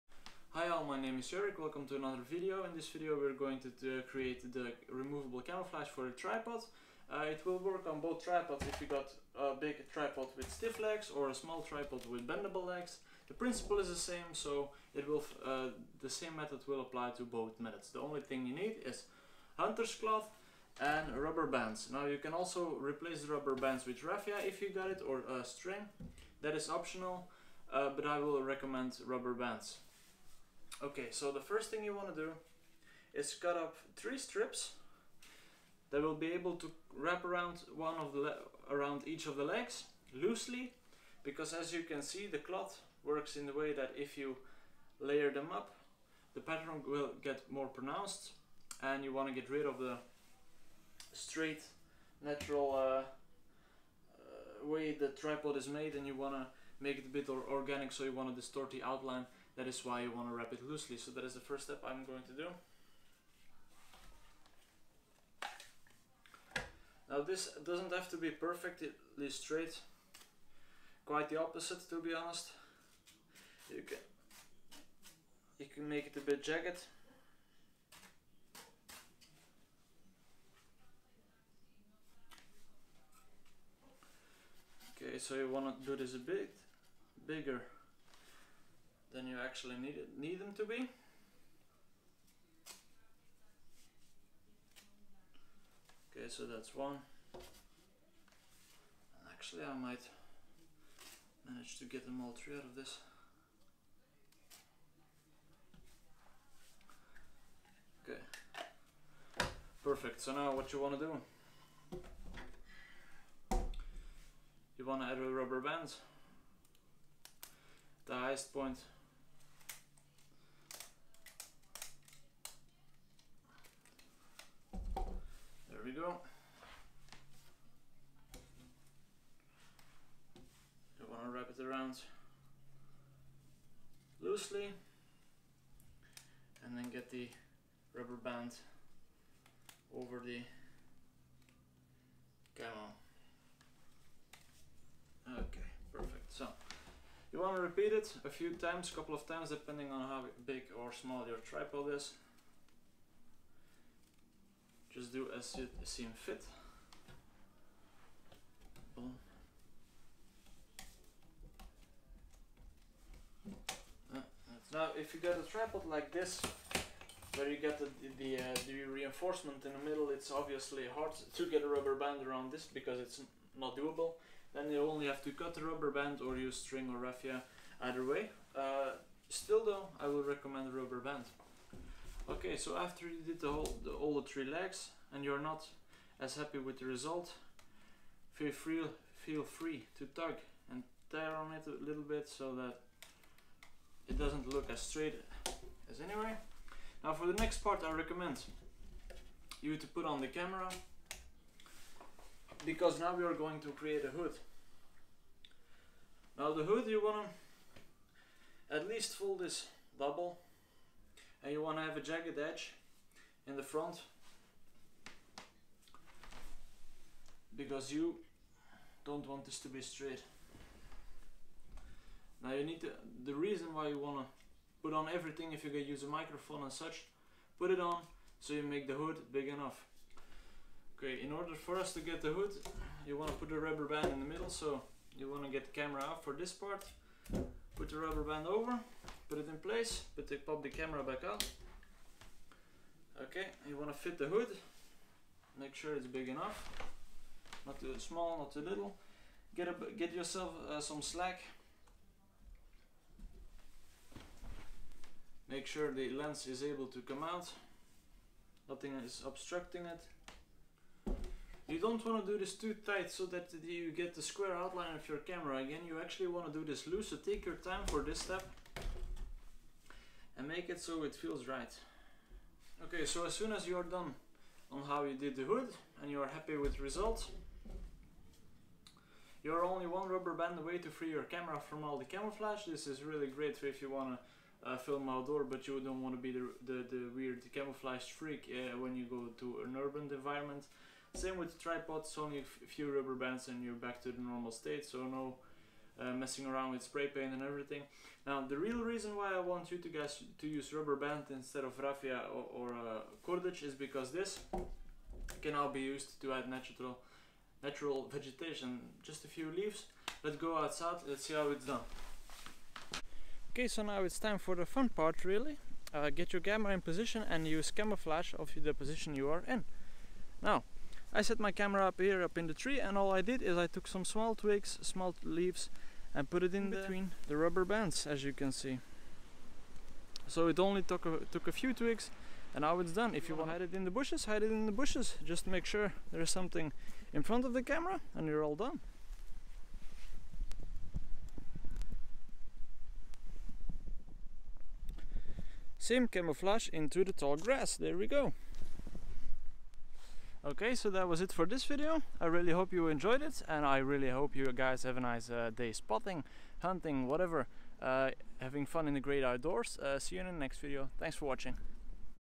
How to cover a tripod with removable camouflage (cheap, fast, easy)
In this video I show how to cover a tripod with camera in REMOVABLE and REUSABLE camouflage. Enjoy!